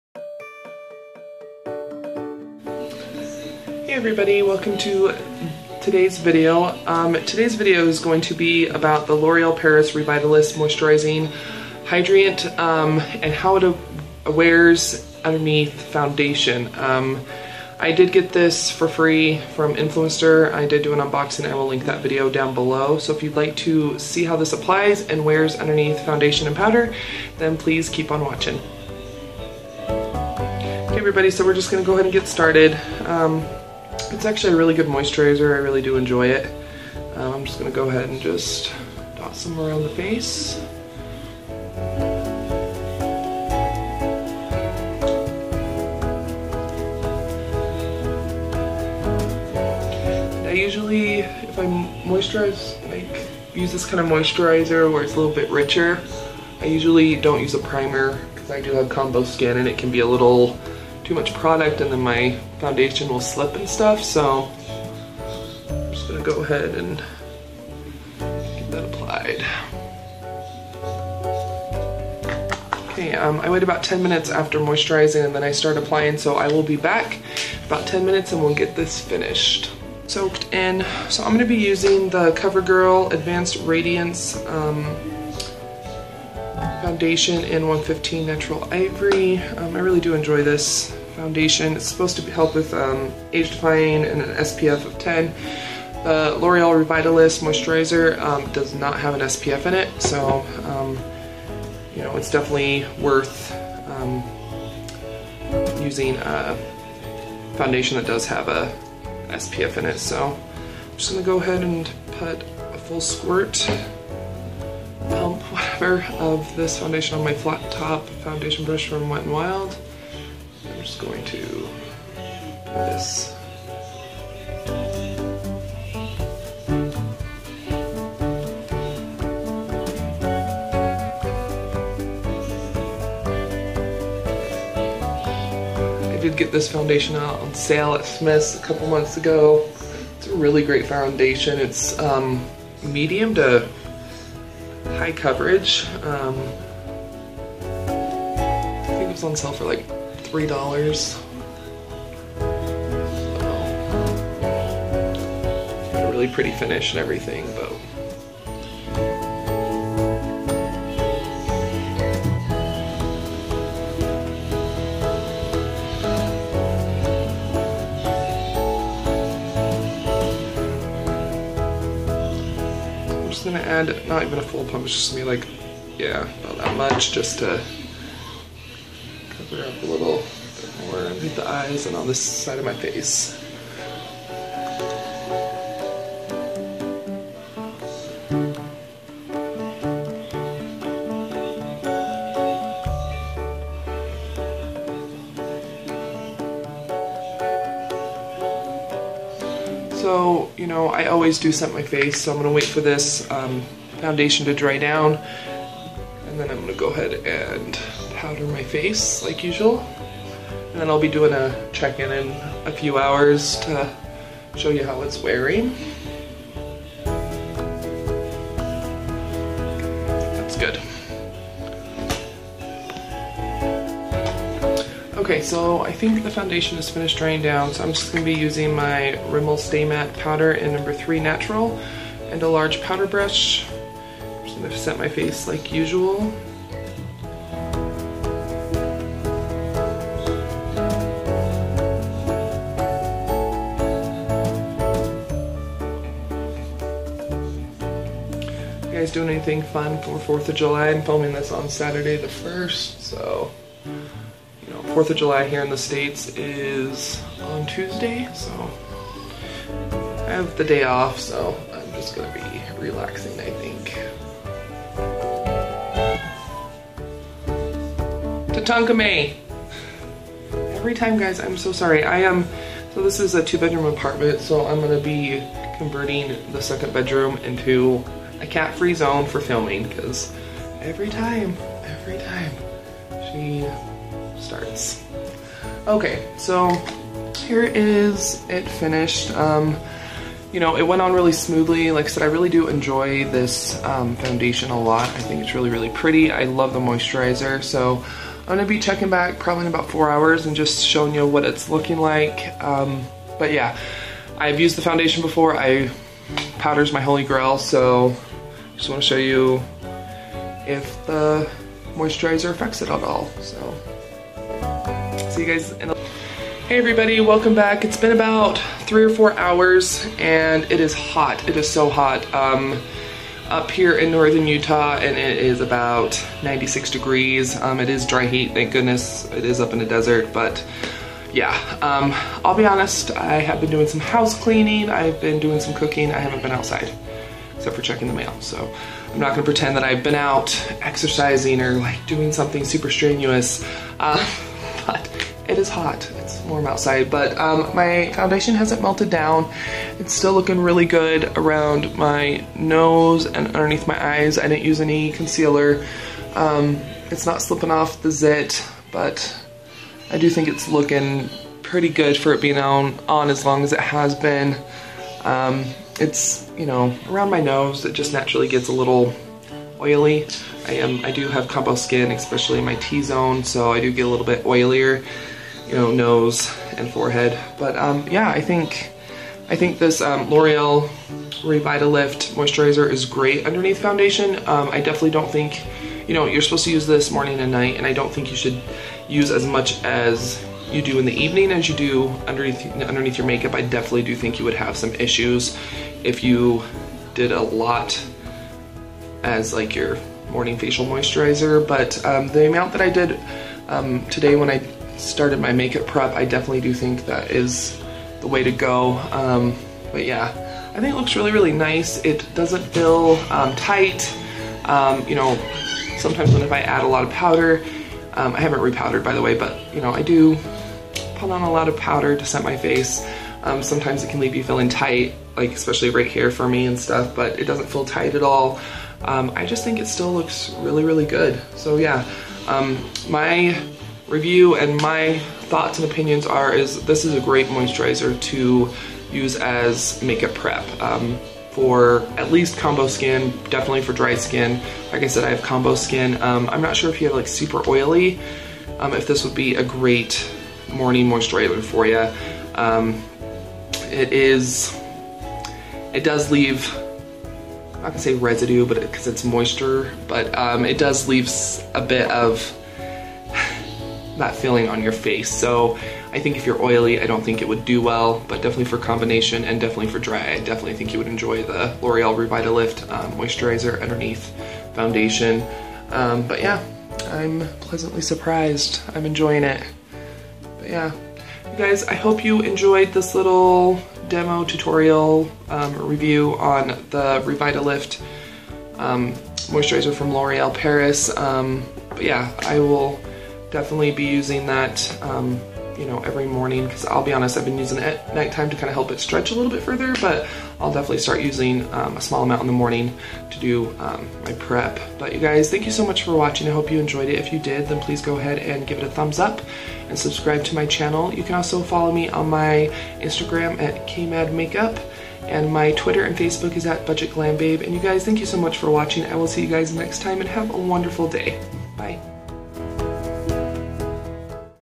Loreal Revitalift Moisturizer, How It Wears Under Makeup
My review and demo of how the Loreal Revitalift Moisturizer wears under foundation and powder. Wear test and check in.